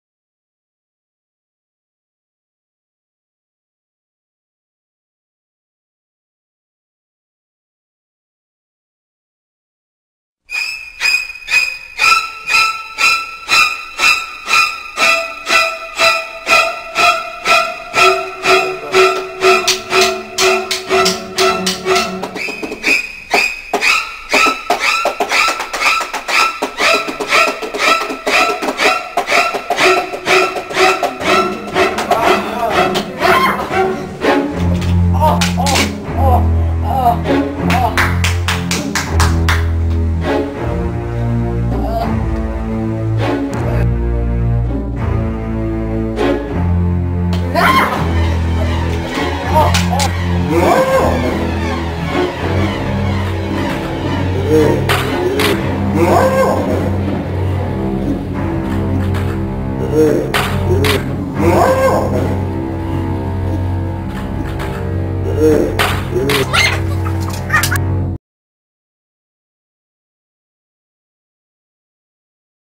Real Scary Ghost Demon
A really scary ghost attacks my cuzin!